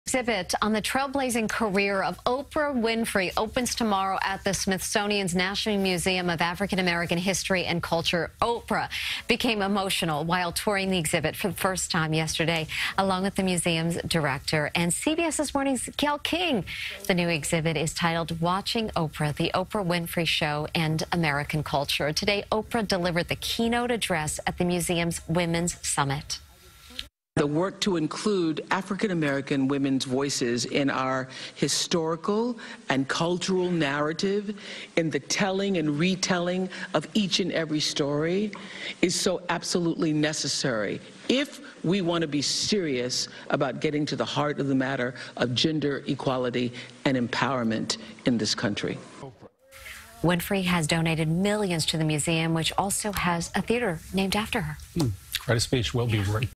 Oprah Winfrey Smithsonian Exhibit
A new exhibit on the trailblazing career of Oprah Winfrey opens Friday at the Smithsonian National Museum of African American History and Culture.